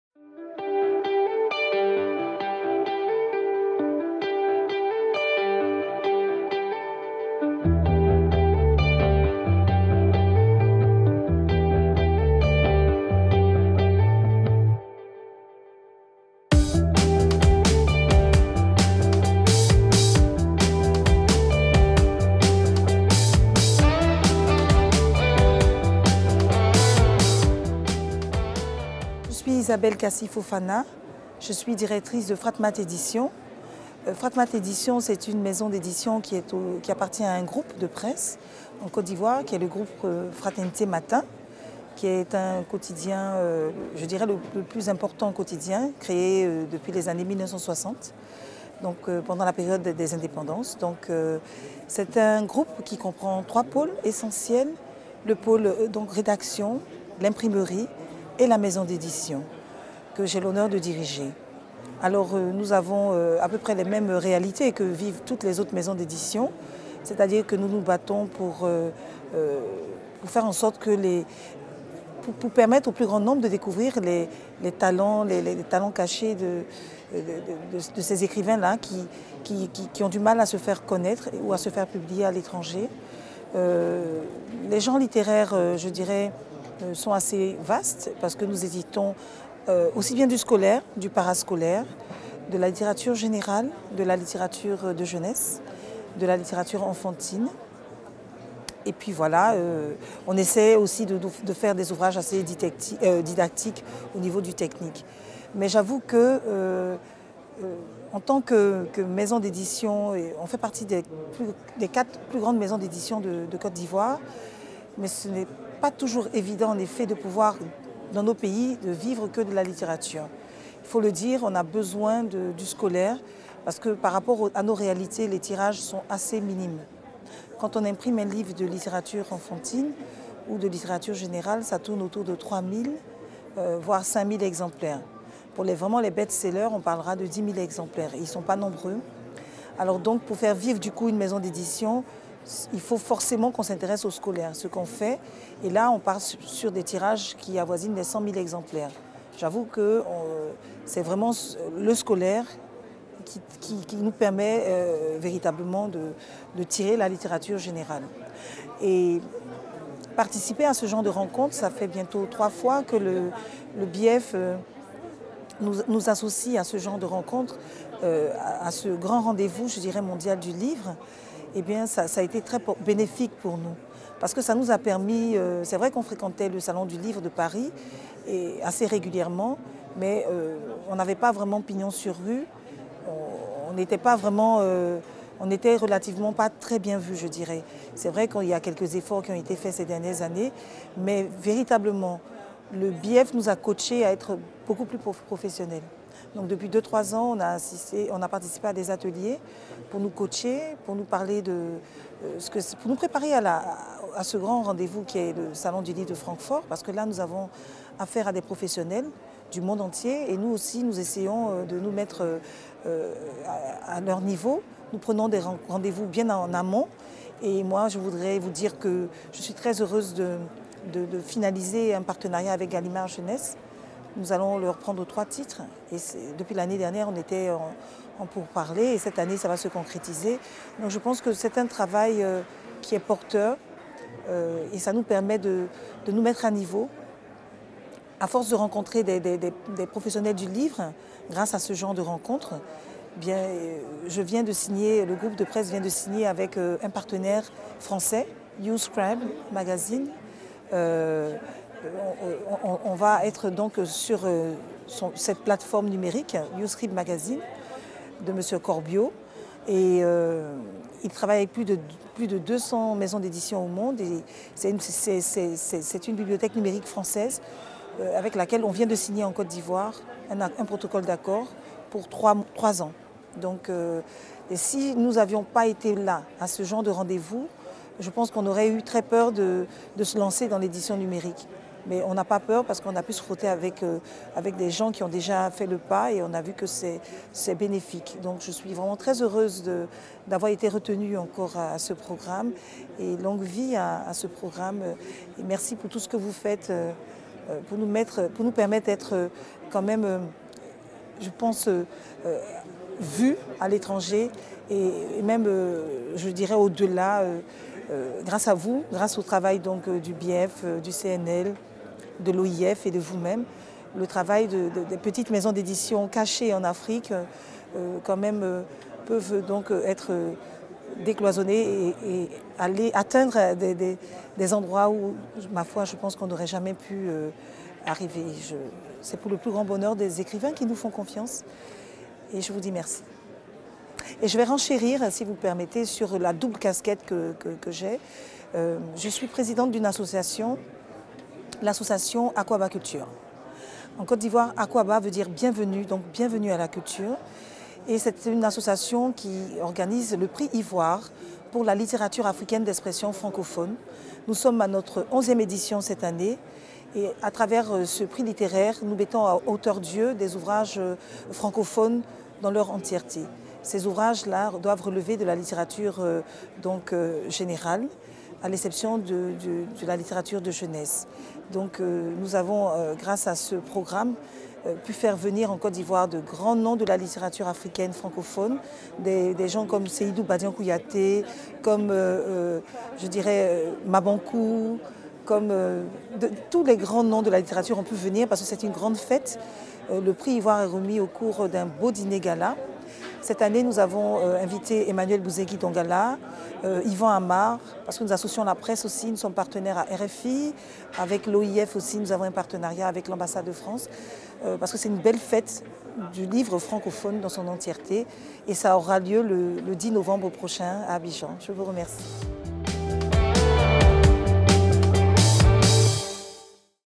Isabelle Kassi Fofana - Directrice de Frat Mat Edition
A l'occasion de la Foire du livre de Francfort 2018, rencontre avec  Isabelle Kassi Fofana, directrice de Frat Mat Edition.